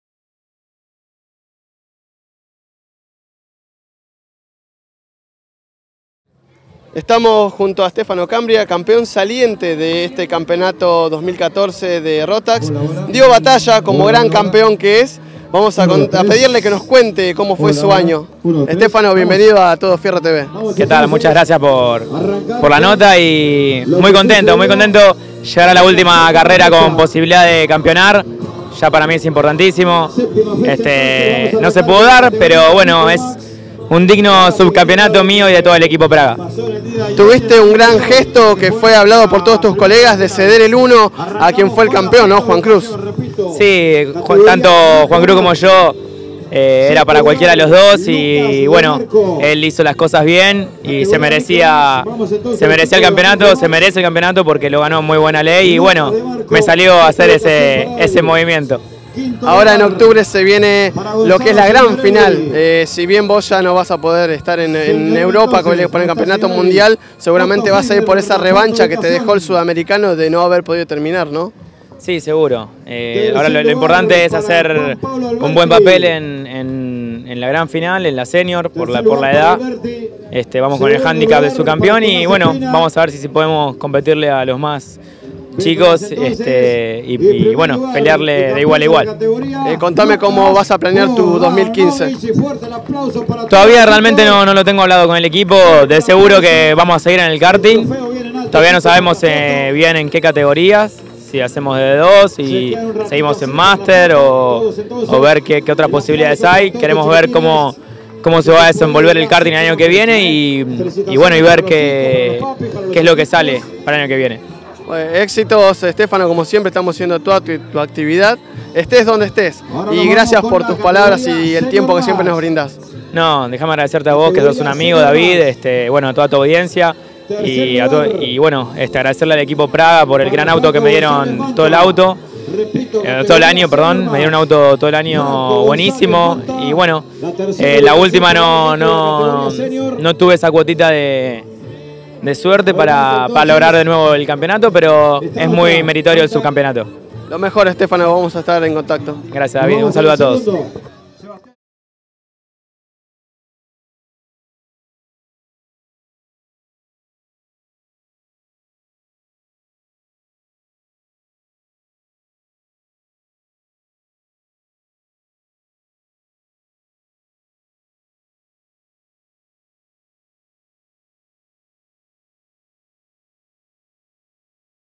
Stéfano Cambia / 7ma. y 8va. Fecha de la Copa Rotax en Ciudad Evita
Stéfano Cambia (Capital Federal)
Master Max
7ma. y 8va. Fecha de la Rotax Max Challengue Buenos Aires.
Kartódromo de Ciudad Evita

Todo Fierro TV
6 y 7 de septiembre de 2014

Daniel Godoy: Director Revista Todo Fierro
Daniel Caamaño: Director Todo Fierro TV
Jeremías Martinez: Camarógrafo & Editor
David Osuma: Periodista
José Aguirre Lamas: Productor